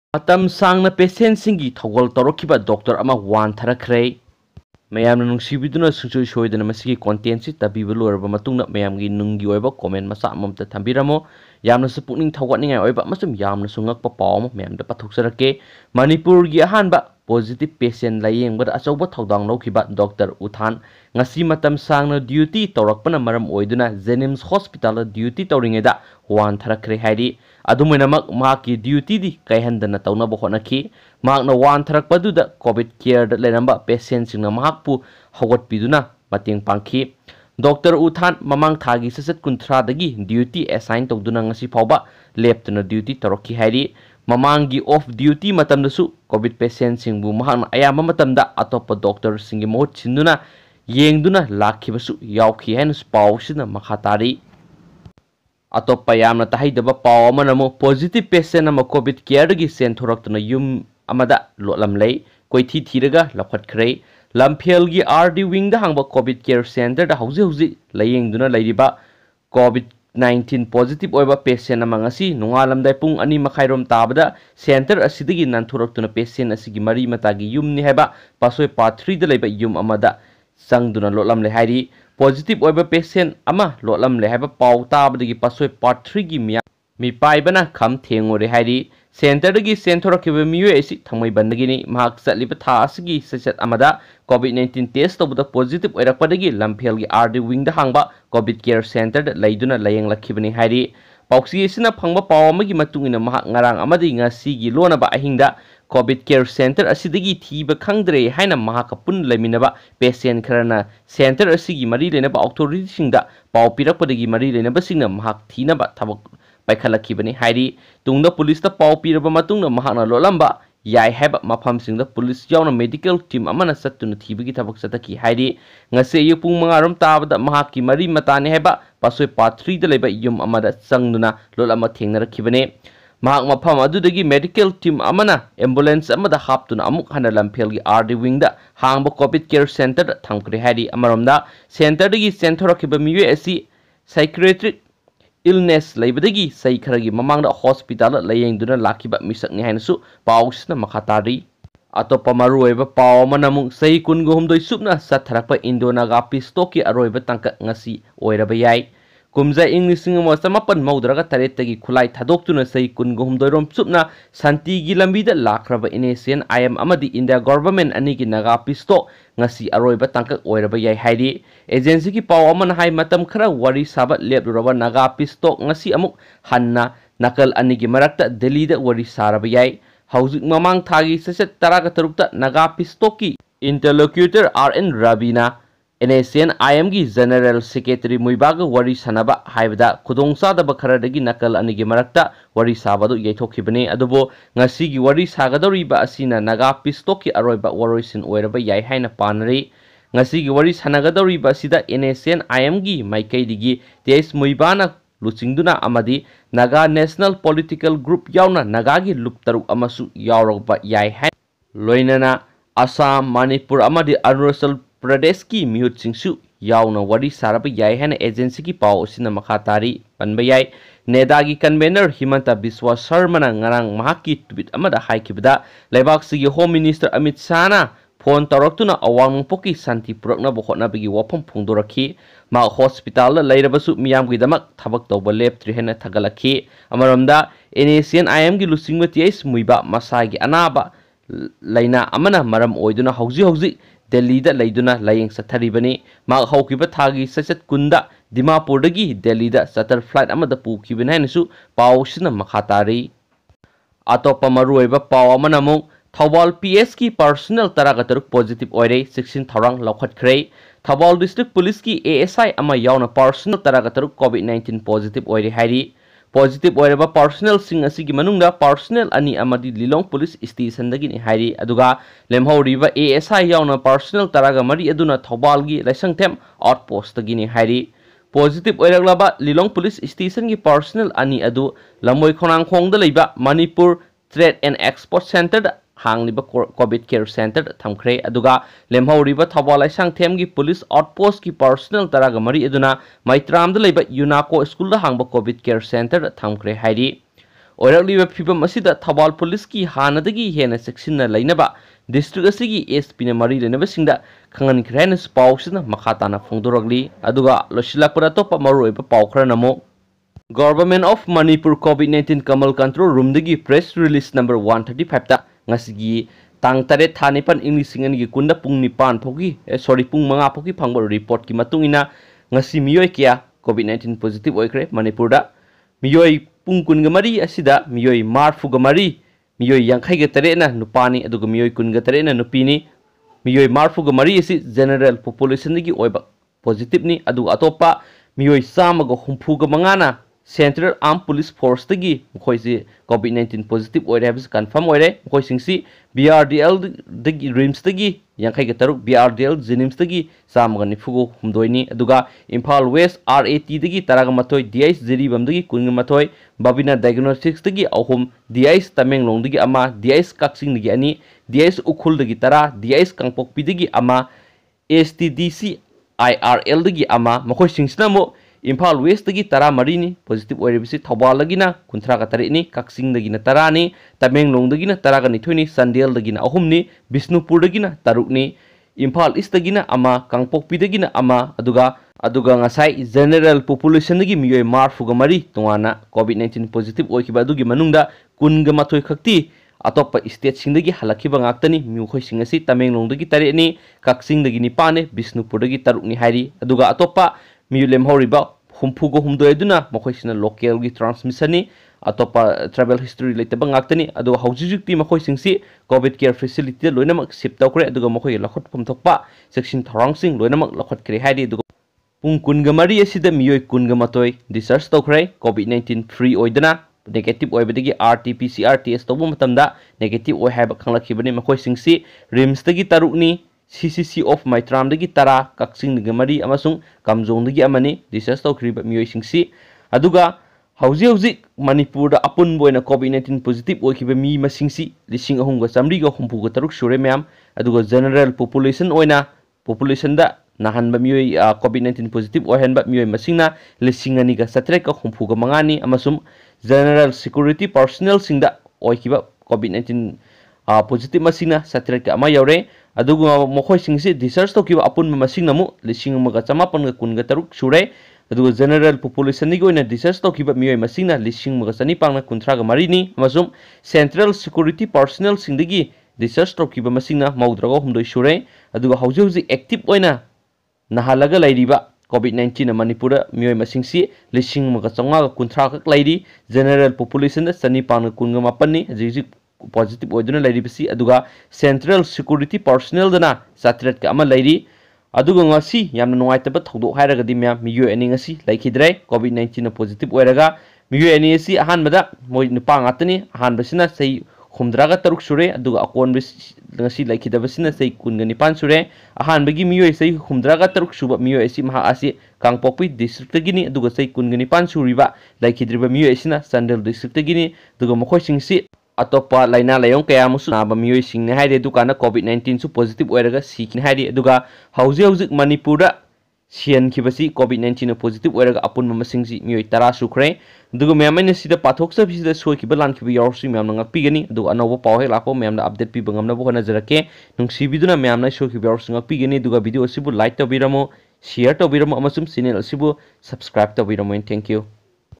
Nongmada punna 249 positive oire 2 sikhre Police 16,CAPF 165 yaona Positive oikhi
Nongmada punna 249 positive oire 2 sikhre Police 16,CAPF 165 yaona Positive oikhi Heiri